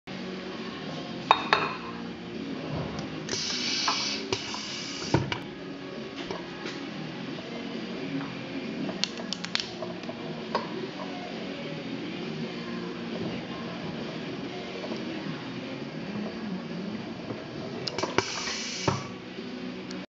Floating air chuck design
Floating air chuck design for minimizing the the effect of topload between the threads.